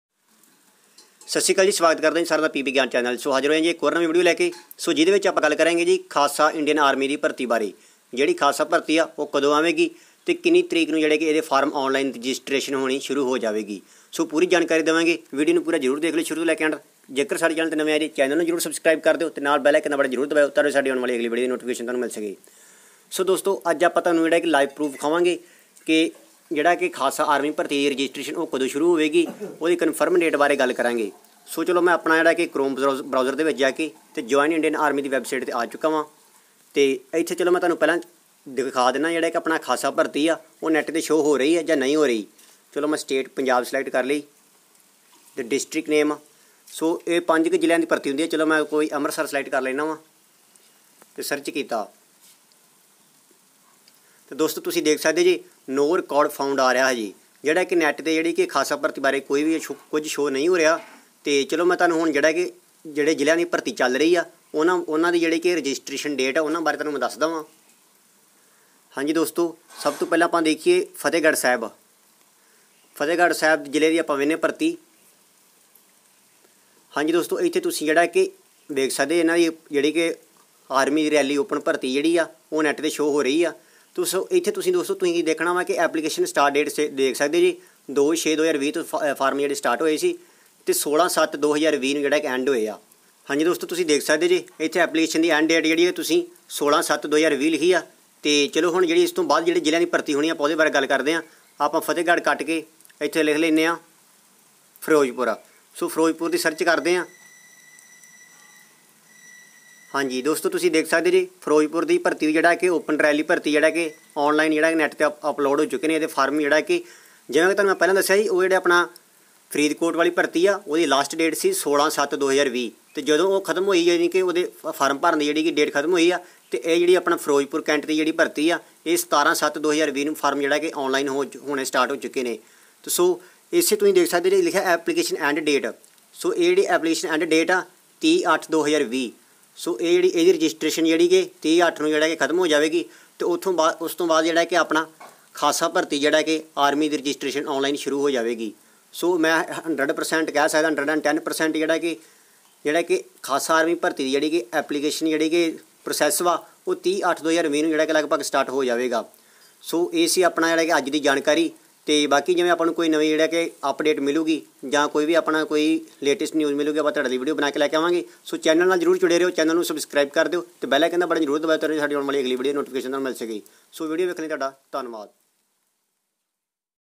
Khasa Army Rally Bharti 2020 | District - Amritsar, Gurdaspur, Khasa, Tarntaran, Pathankot
Postponed Army Rally Bharti 2020 | District - Amritsar, Gurdaspur, Khasa, Tarntaran, Khadur sahib |